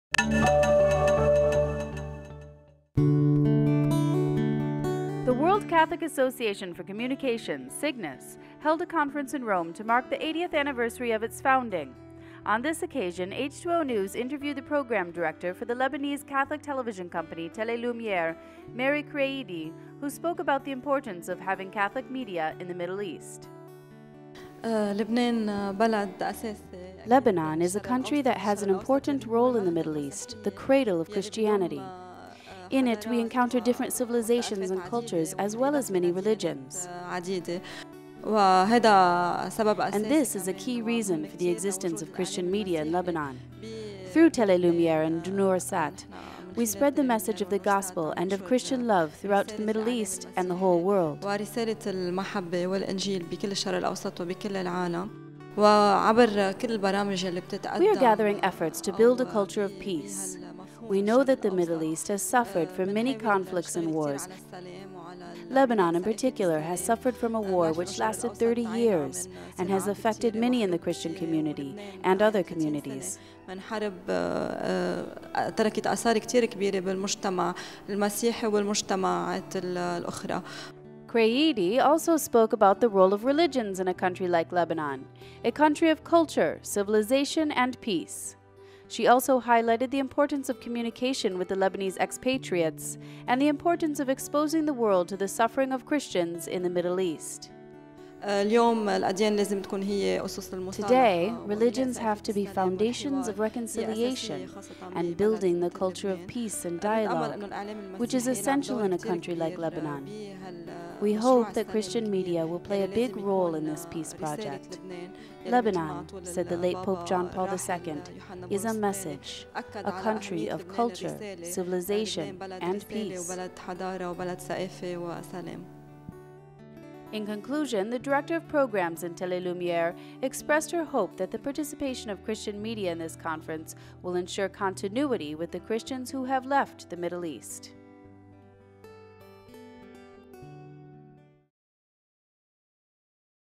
Communications and the Culture of Peace
The World Catholic Association for Comminication, Signis, held a conference in Rome to mark the 80th anniversary of its founding. On this occasion H2onews interviewed the program director of  the Lebanese Catholic television company, Tele Lumiere, Mary Kreidy, who spoke  about the importance of having Catholic media in the Middle East.&nbsp;  &quot;Lebanon is a country that has an important role in the Middle East, the cradle of Christianity. In it we encounter different civilizations and cultures, as well as many religions.And this is a key reason for the existence of  Christian media in Lebanon. Through Tele Lumiere and Noursat we spread the message of the Gospel and of Christian love throughout the Middle East and the whole world.We are gathering efforts to build a culture of peace. We know that the Middle East has suffered from many conflicts and wars, Lebanon in particular has suffered from a war which lasted thirty years and has affected many in the Christian community and other communities.&quot;Kreidy also spoke about the role of religions in a country like Lebanon, a country of culture, civilization and peace. She also highlighted the importance of communication with the Lebanese expatriates and the importance of exposing the world to the  suffering of Christians in the Middle East.&quot;Today, religions have to be foundations of reconciliation and building the culture of peace and dialogue, which is essential in a country like Lebanon. We hope that Christian media will play a big role in this peace project. Lebanon, said the late Pope John Paul II, is a message, a country of culture, civilization and peace.&quot;In conclusion, the director of programs in Tele Lumiere expressed her hope that the participation of Christian media in this conference will ensure continuity with the Christians who have left the Middle East.